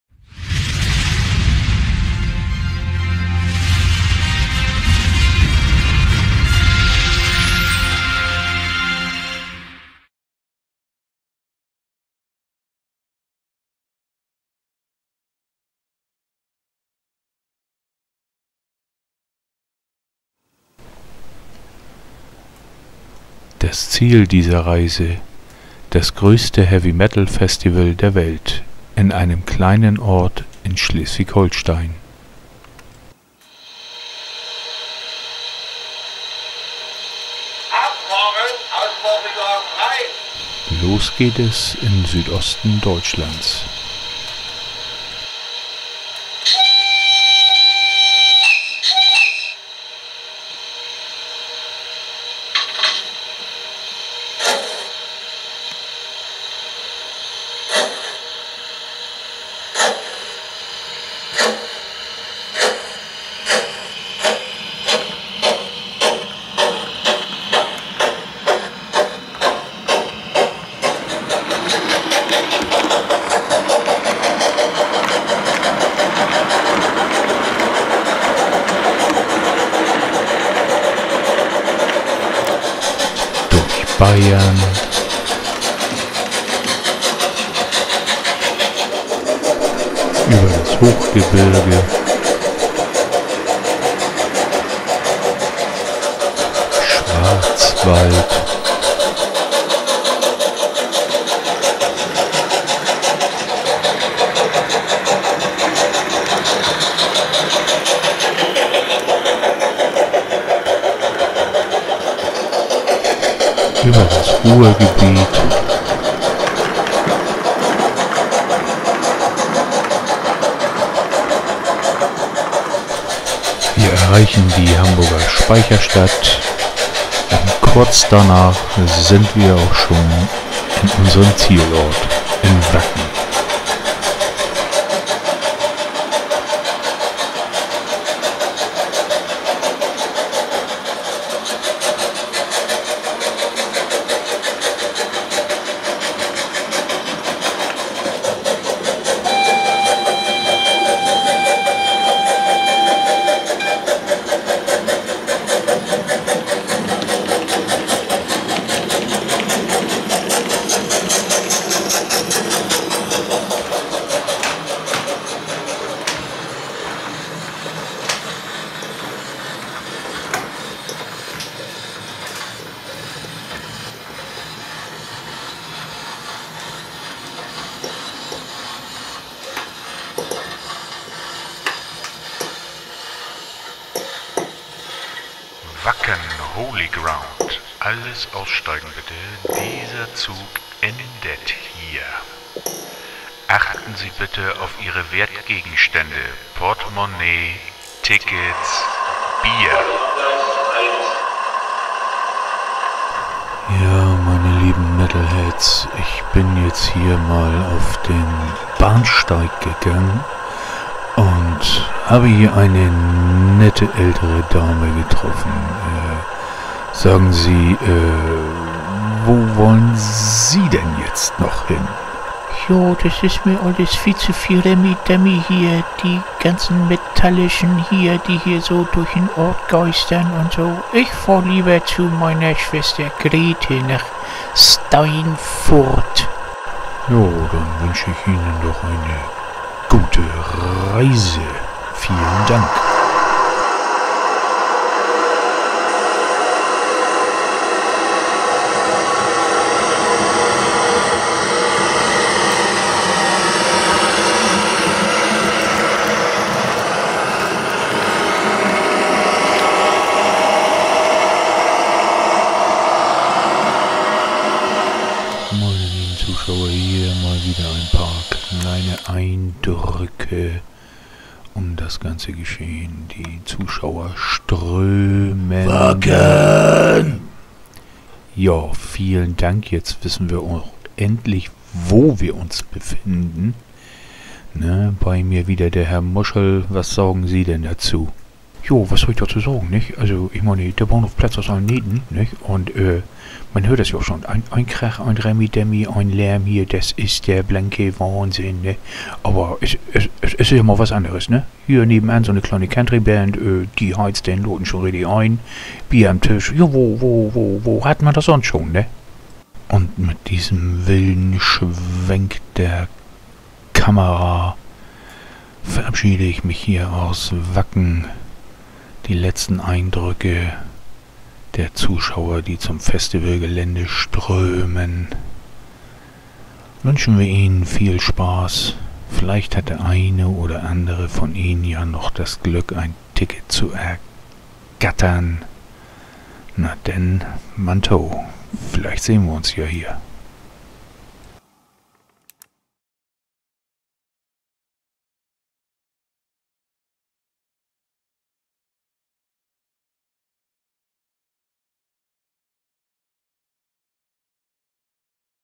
Mit dem Heavy Metaltrain nach Wacken Spur0
Ein Film, gedreht auf meiner Spur 0 Eisenbahnanlage. Erzählt wird die Geschichte von einem Zug der quer durch die Republik nach Wacken fährt.
Wer mehr über diese Anlage und deren Entstehung wissen möchte, schaut am besten auf meiner Homepage vorbei